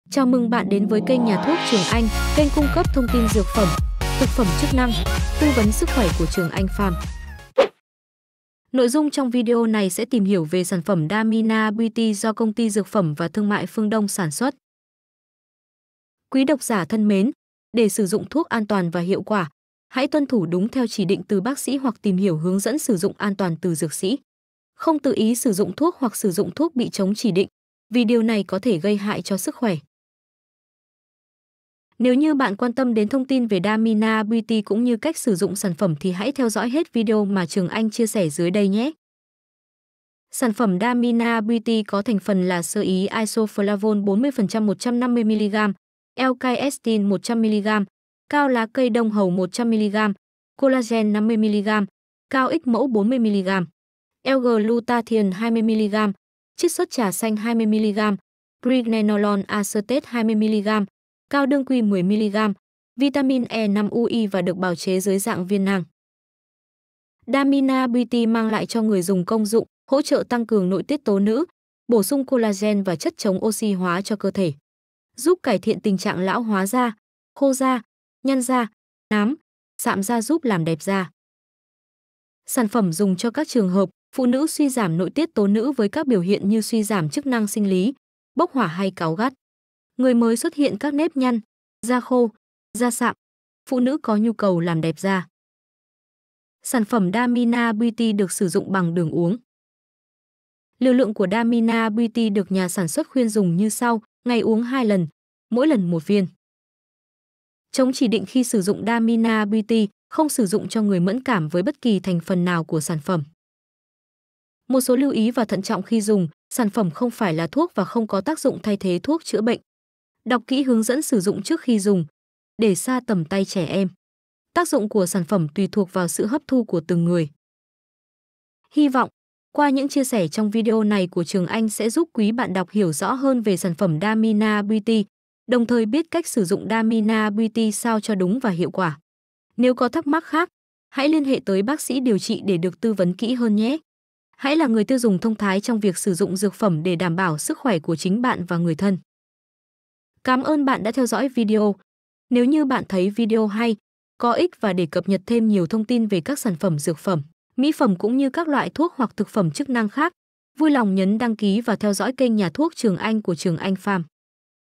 Damina Beauty - Hỗ trợ tăng cường nội tiết tố nữ - Quaythuoctruonganh
Công dụng
Hỗ trợ tăng cường nội tiết tố nữ, bổ sung collagen và chất chống oxy hóa cho cơ thể. Hỗ trợ cải thiện tình trạng lão hóa da, khô da, nhăn da, nám, sạm da và hỗ trợ làm đẹp da.